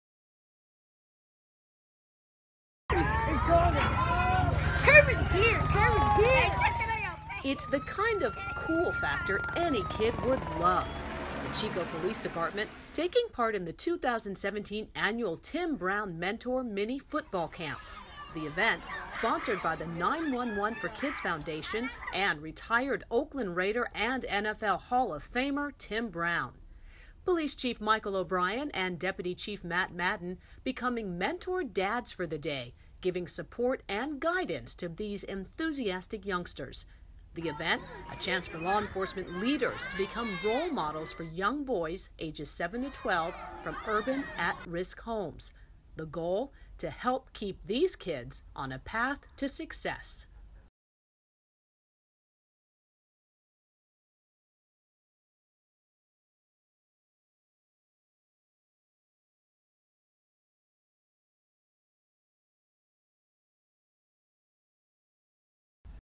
Chico Police Department Mentoring Kids with NFL Star Tim Brown
The Chico Police Department participates joins retired NFL Oakland Raider and NFL Hall of Fame player Tim Brown for the annual Mentor Mini-Football Camp, April 21, 2017.
The event is a one day football camp for young boys between the ages of 7-12 from urban, single parent, fatherless homes.
Law enforcement leaders serve as role models and mentor dads for the day, as the youngsters learn to interact through football training drills.
The camp also provides an opportunity for older, more experienced individuals to provide guidance and support to a minor whose environment may increase his chances of academic failure and exposure to harmful societal factors.